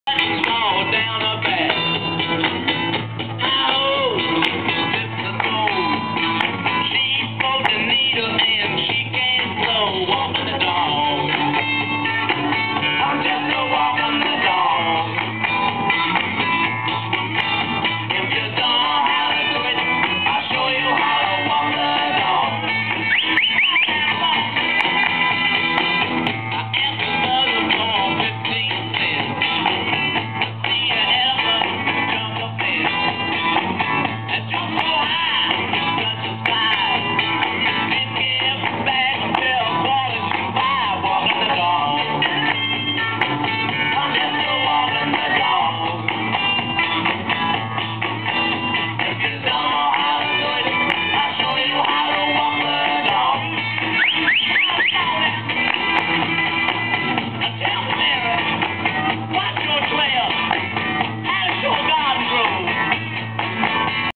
Outdoor British Fashion Catwalk Show, London. July 2013. Buckingham Palace Gardens.
Queen's Coronation Festival at Buckingham Palace Gardens, 2013.